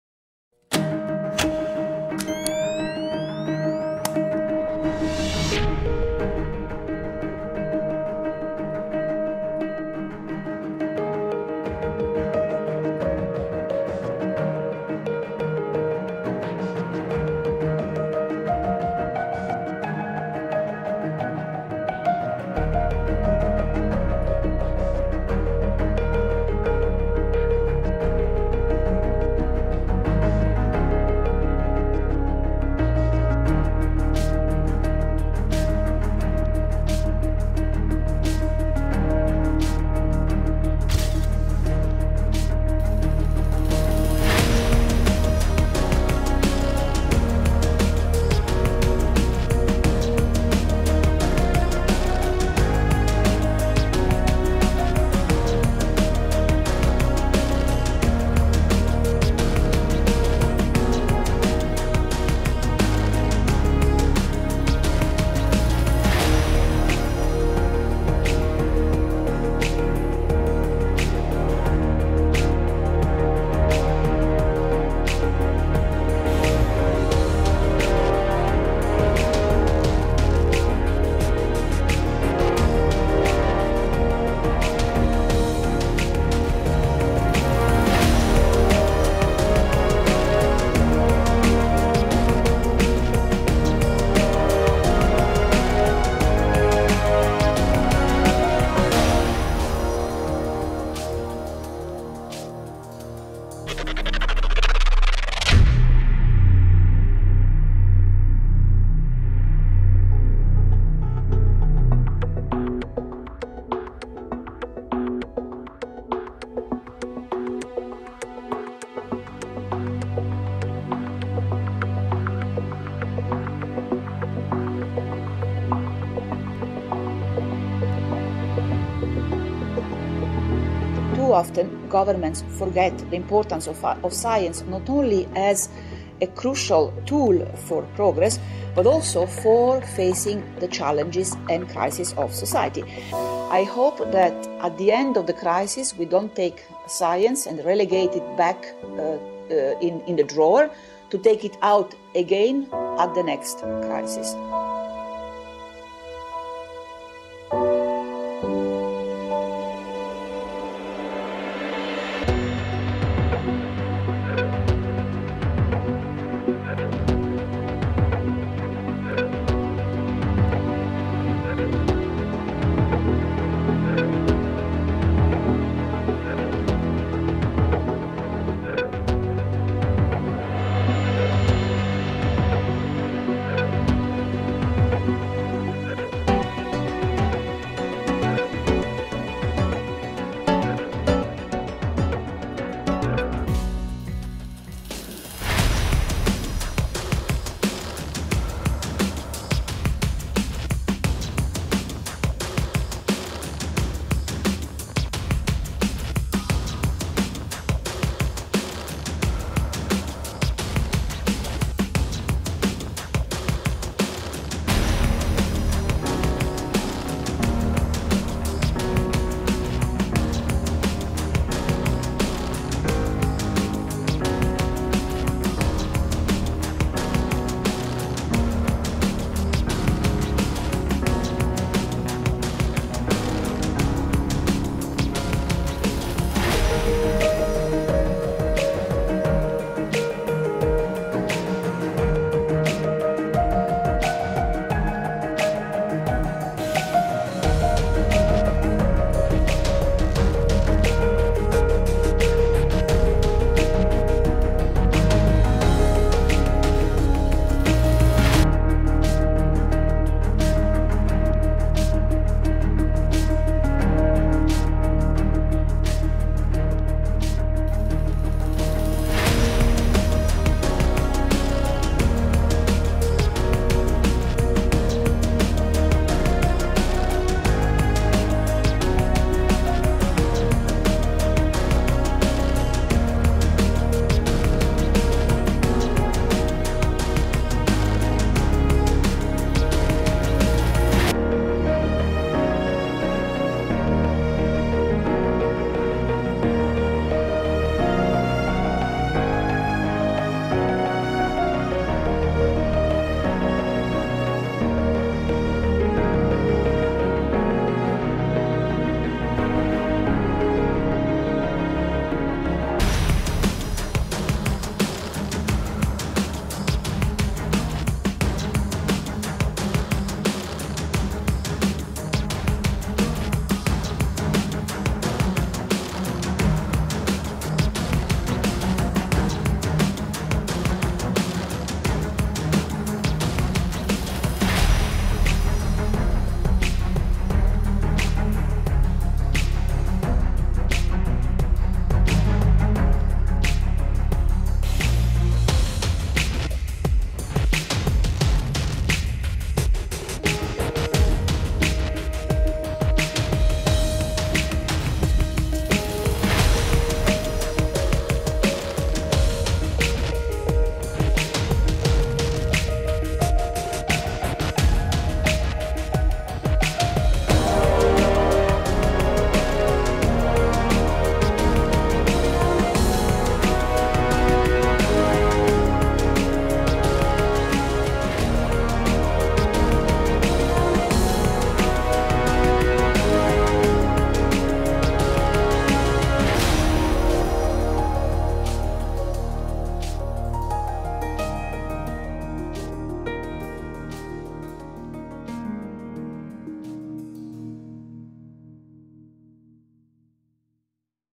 CERN Highlights 2020
A video showing the major milestones at CERN in 2020.